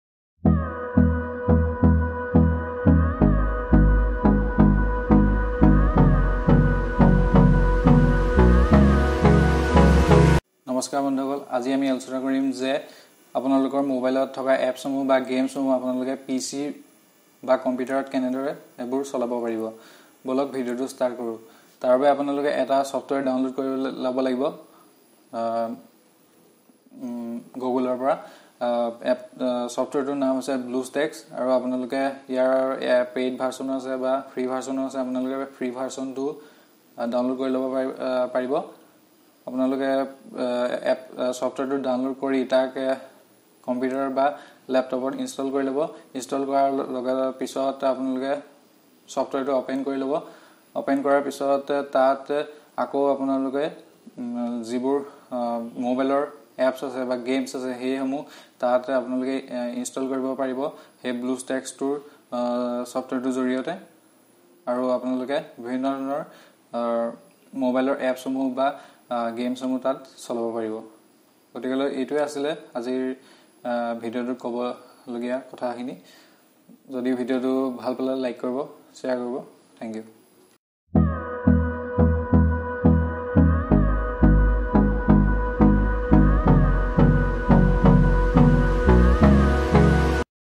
HOW TO PLAY ANY ANDROID APPS IN COMPUTER / LAPTOP||IN ASSAMESE|| bjd tech
software name BLUESTACKS
Download free version from google
Fb Biplob Jyoti Dutta
thanks again :)